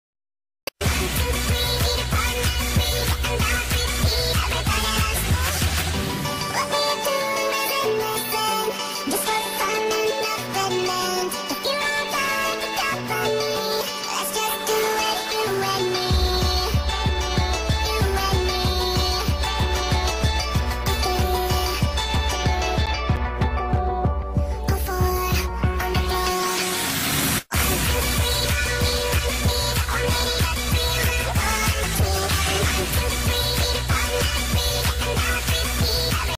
One Two Three (Meme)
This took forever to finish, stupid windows movie maker kept freezing again...

Oh well this came out great besides the fact I took off a few seconds. XDD I did not come up with the original idea for this. Enjoy! X3

Cats shown:

Courtney - Me
Fuzzymask - Ginathehedgehog
Chris - Exammaster
Victoria - DuckyPrincess728
Aaron - AaronxGwen31
Lumina - Belongs to Ginathehedgehog
Shadow - Belongs to Ginathehedgehog
Phantom - Belongs to Ginathehedgehog
My mom. XDD